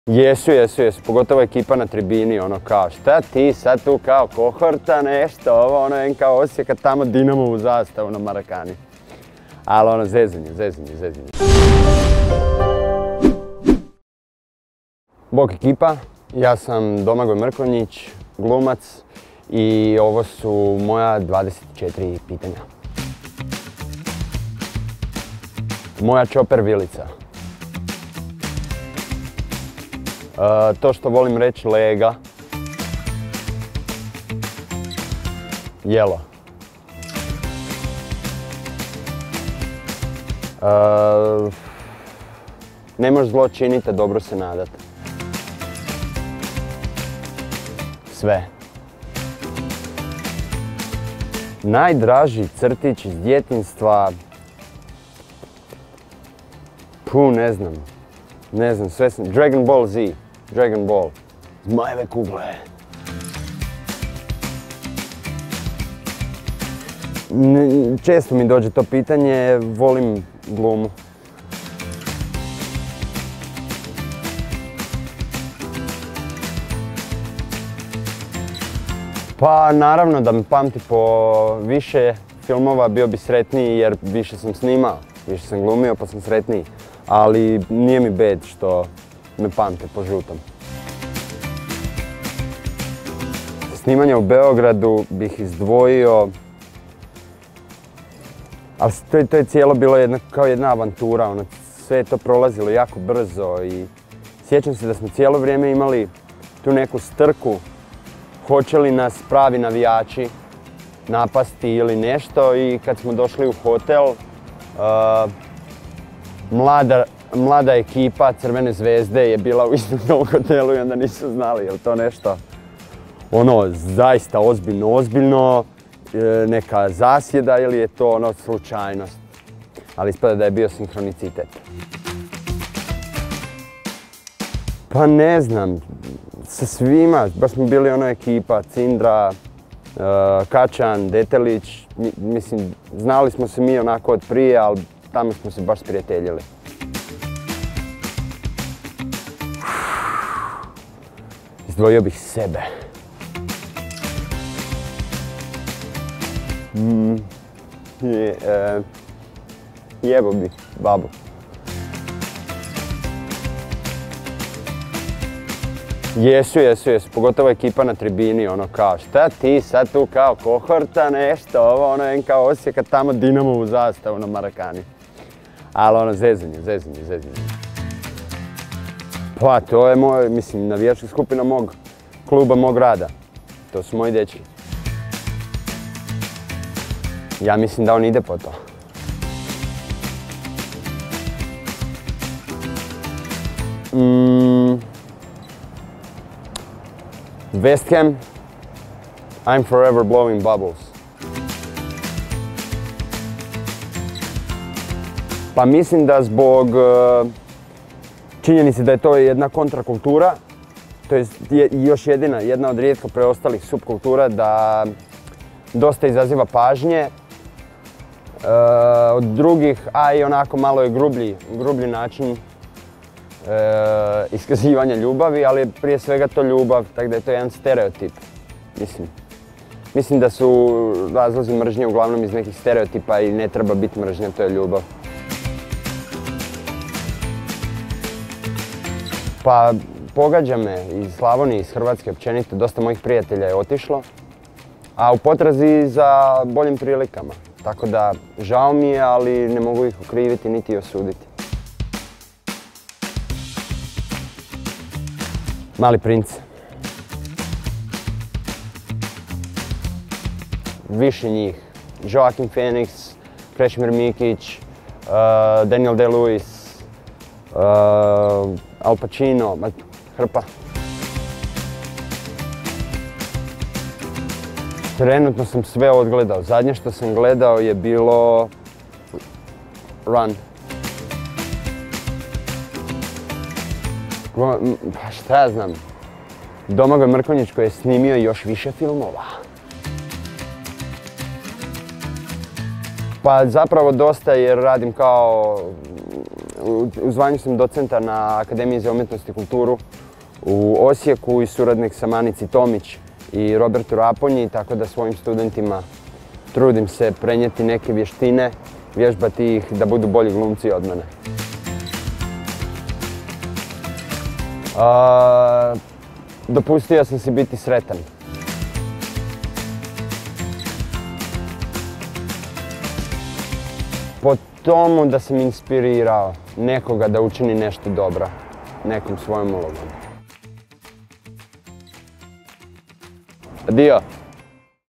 Žuti iz filma 'ZG80': 'Kohorta me zezala jer sam nosio Dinamovu zastavu' | 24 pitanja
Osječanin koji je glumio Bad Blue Boysa gostovao nam je ju 24 pitanja. Domagoj Mrkonjić otkrio je pozadinske priče iz Kohorte i snimanja u Beogradu, te anegdotu s navijačima Crvene Zvezde...